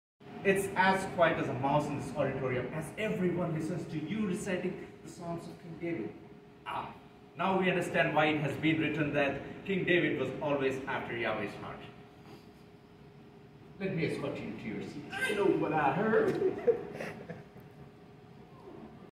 Rehearsal Take -
Will Wade and Tasmay rehearse a scene from Act 1 of 4 Daughters Minus One, 2000 A.D.